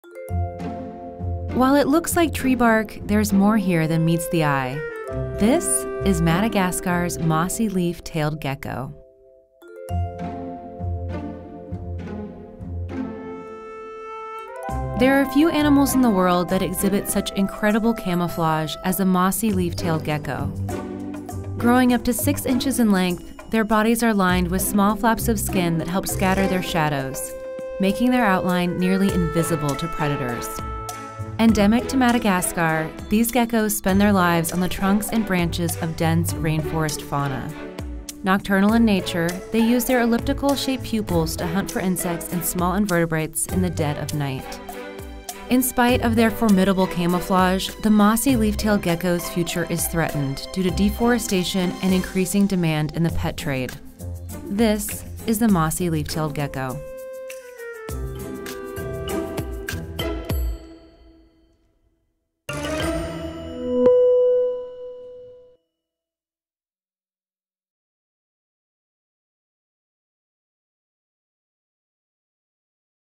The Amazing Camouflage of the Mossy Leaf-Tailed Gecko
This is Madagascar’s mossy leaf-tailed gecko. With the ability to grow up to six inches in length, these creatures possess an incredible camouflage that allows them to blend right into their forest habitats. They can scatter their shadows, making their outline nearly invisible to predators. Unfortunately, due to deforestation and increasing demand in the pet trade, the future of the species is under threat.